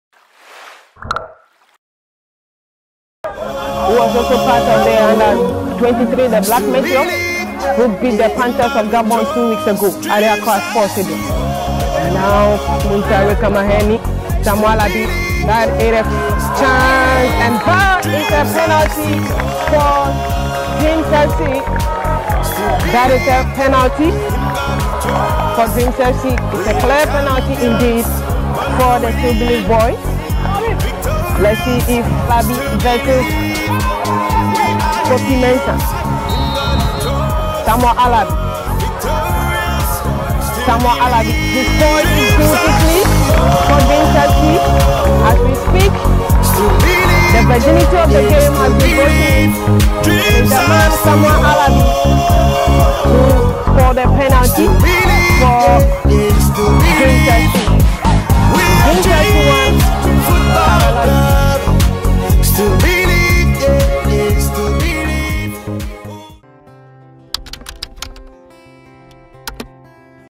Dreams 1 - 1 Karela [ S Alarbi grabs first league goal]
Match day 2 - GFA SPECIAL COMPETITION
VENUE: THEATRE OF DREAMS, DAWU